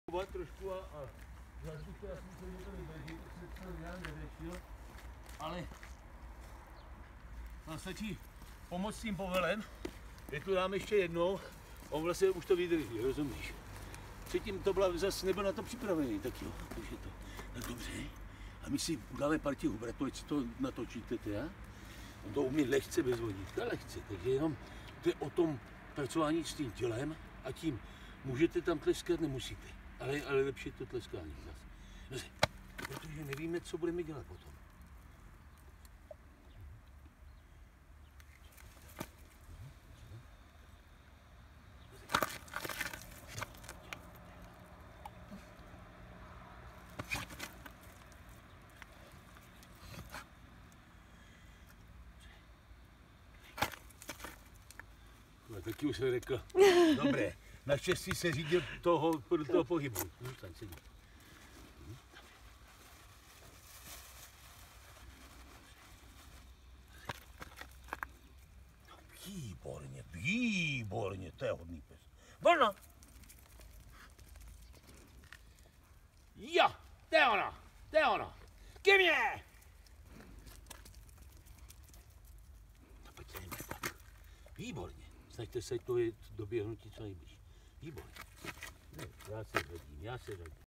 IMG 6524 Ukázka tréninku Dl paměti, při „předávačce“
Morgan byl při příchodu dost bujný kamzík, který si nenechal nic líbit a jakákoliv fyzická manipulace byla nebezpečná a proto nemyslitelná.
Na snímku jsem majitelům ukázal, jak má Morgan vybudovanou tzv.: „DLOUHODOBOU PAMĚŤ“ a vysvětlil, jak je to při hře, případně při výcviku výhodné využívat a proč.